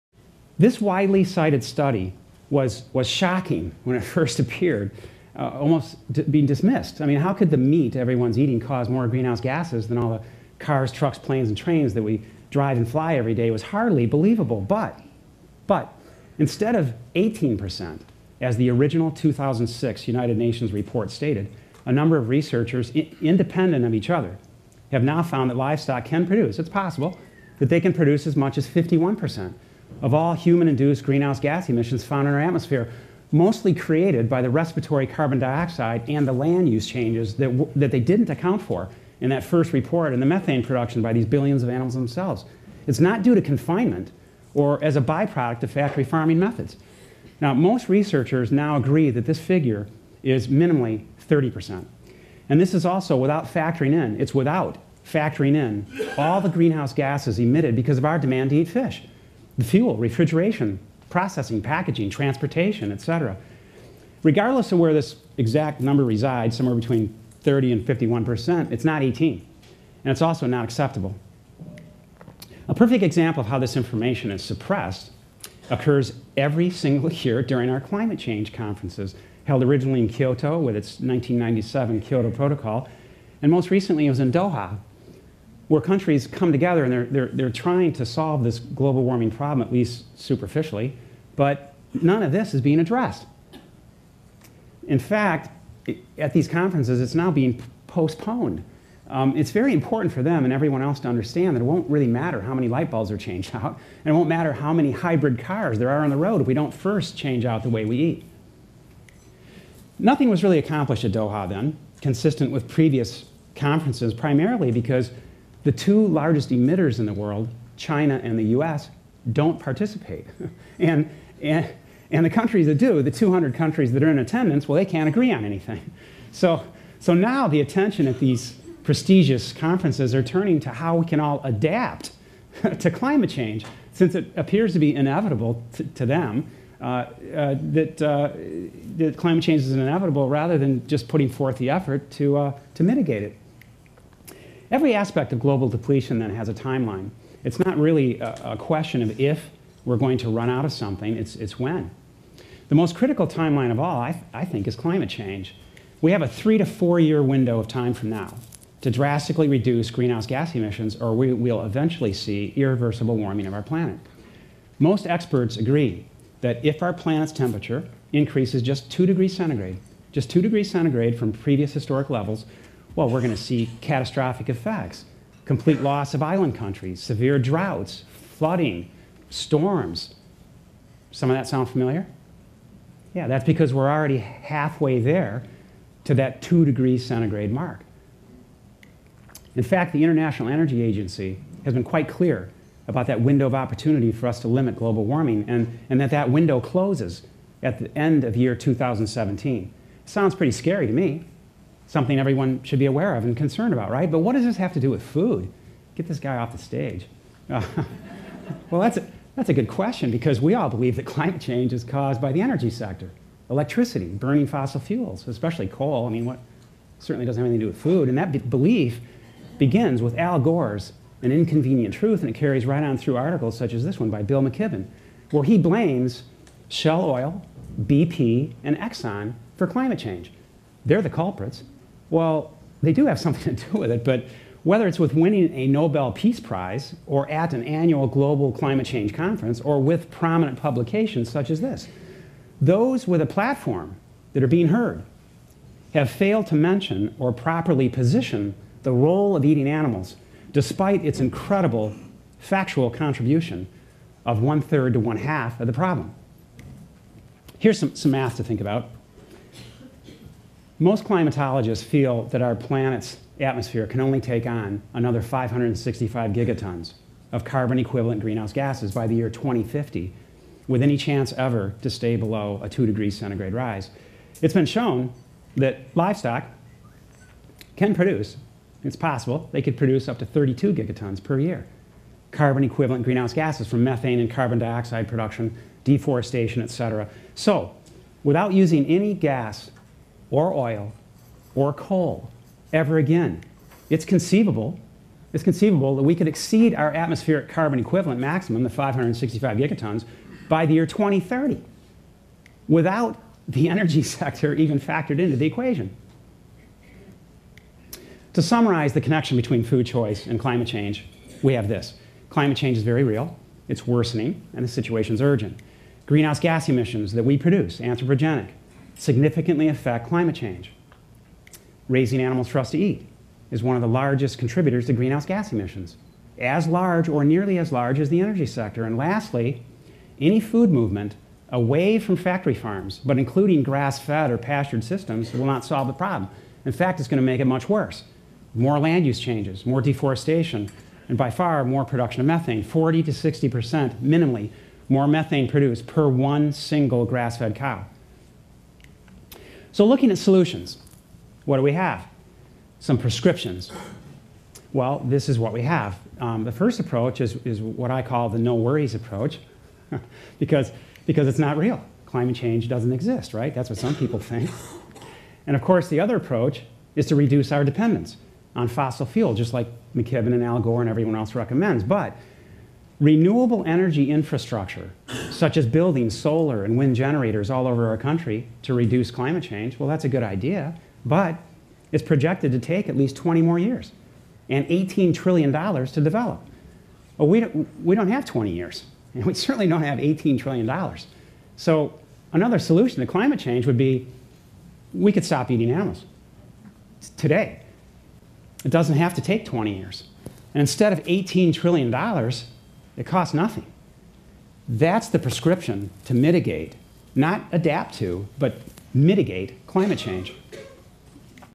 Green house gasses, climate change, and food choice. What you can do.
Dr. Oppenlander explains how livestock contributes to the emission of greenhouse gasses. He also describes how this valuable information is suppressed by conferences held to discuss climate change.  He provides a timeline and prescription of climate change issues and connects how it all relates to food choice.